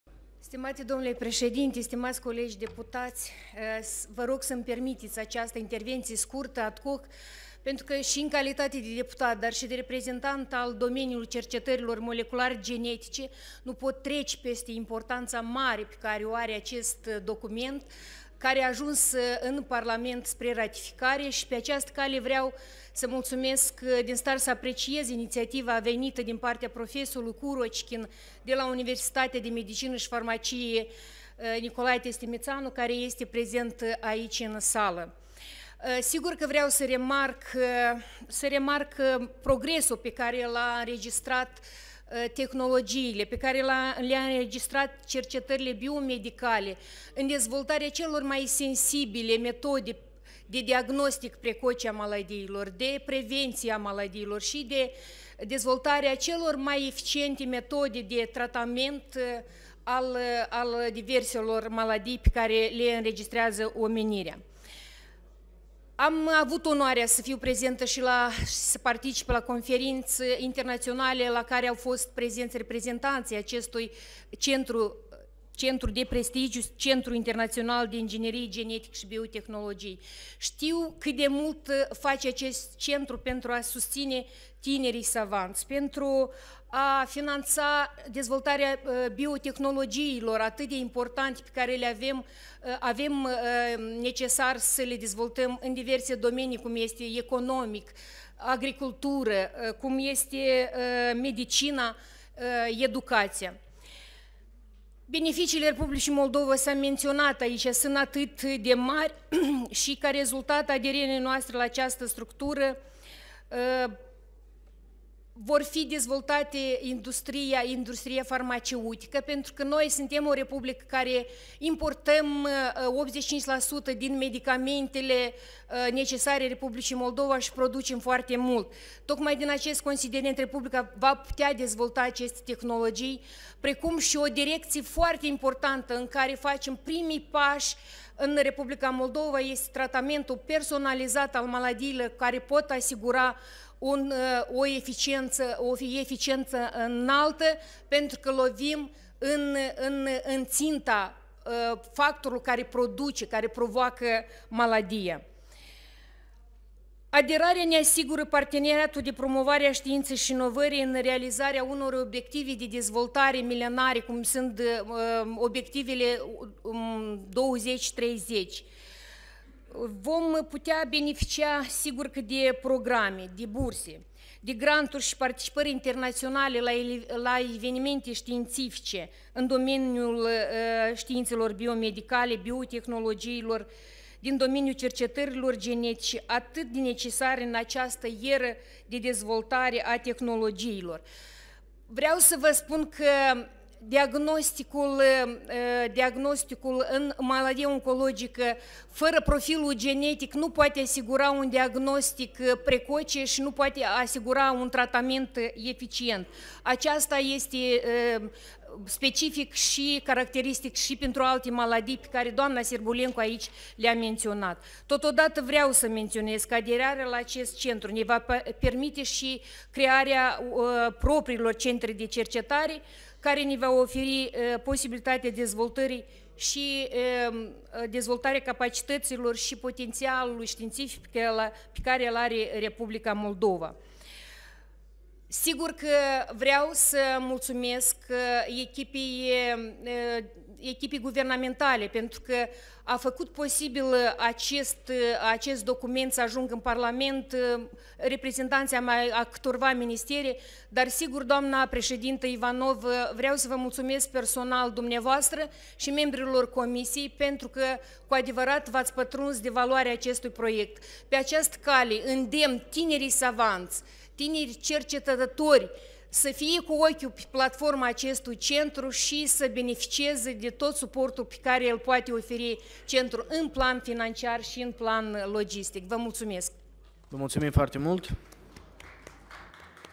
Aderarea RM la statutul centrului internațional de inginerie genetică și biotehnologie
Valentin Stratan despre aderarea Republicii Moldova la statutul centrului internațional de inginerie genetică și biotehnologie. Puteți privi ședința integrală aici